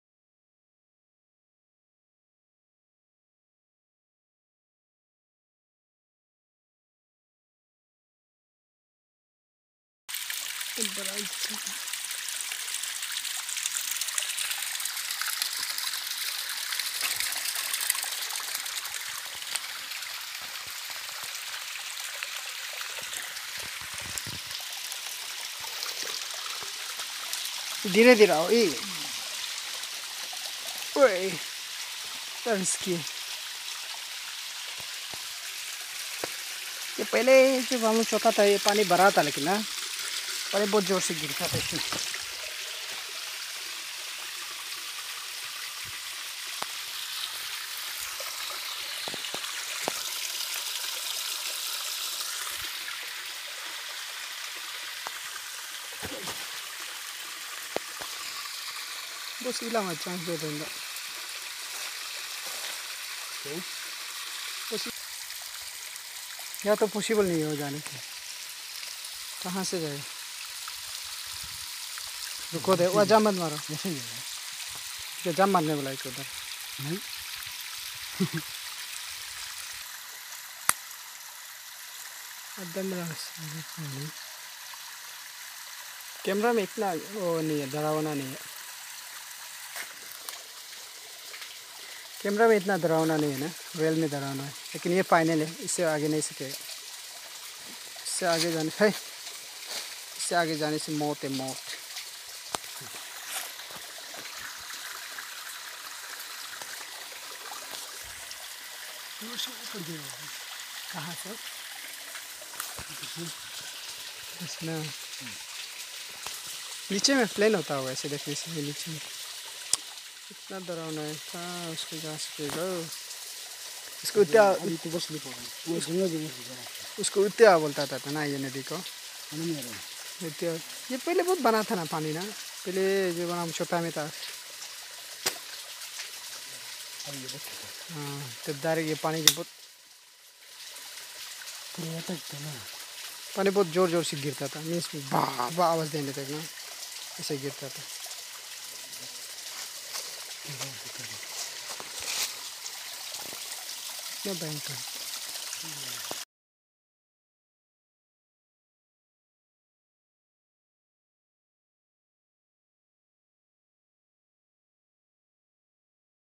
Nature of beautiful water falls in Kamle dstrct A.p
we are found in jungle for during the hunting something eatable insect